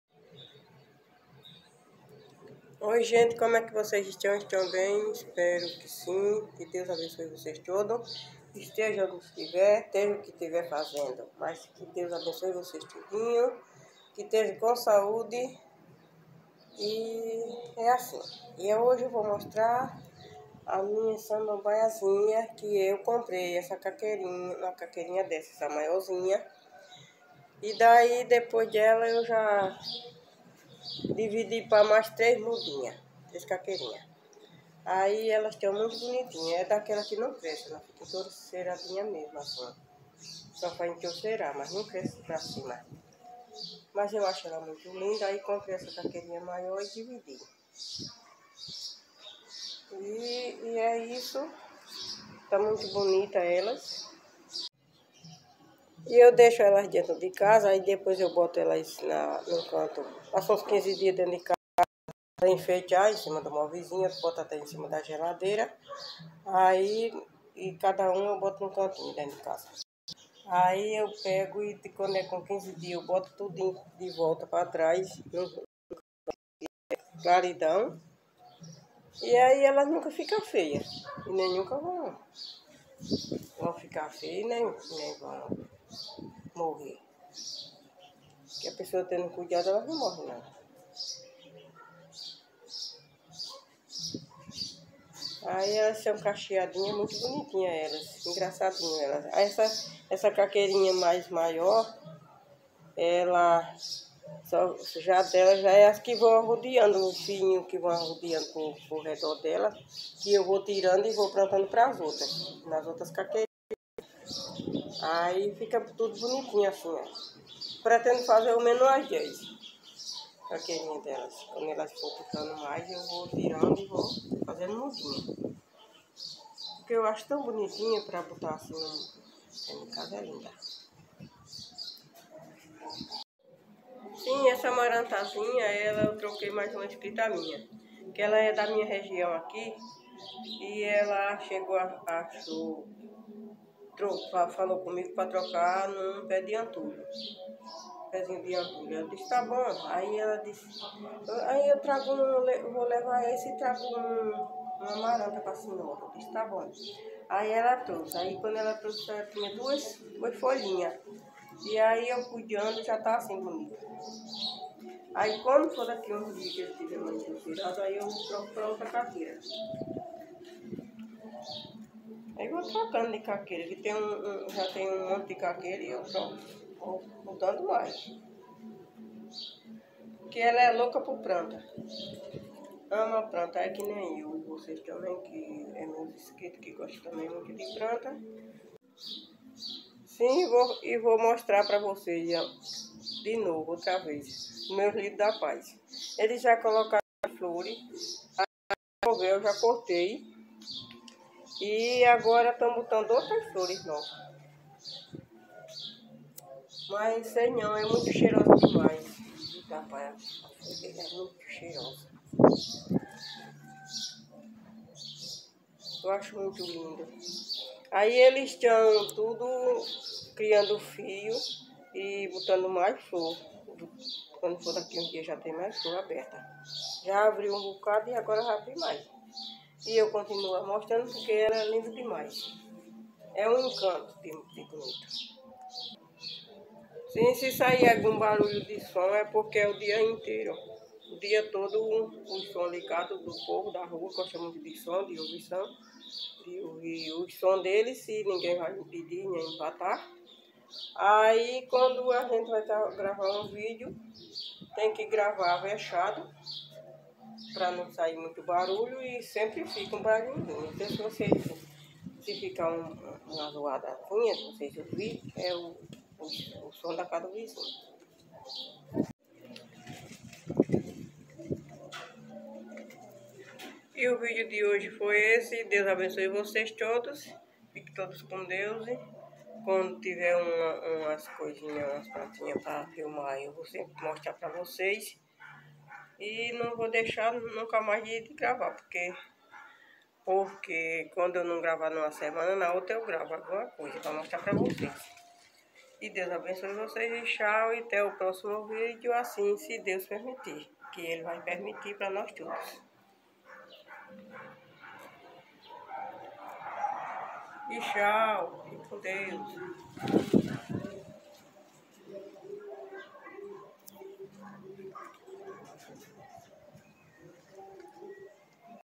Aumentei a mini samambaia e mais
Olá, gente, espero que estejam todos bem. Propaguei minha mini samambaia e mostrei mais plantas.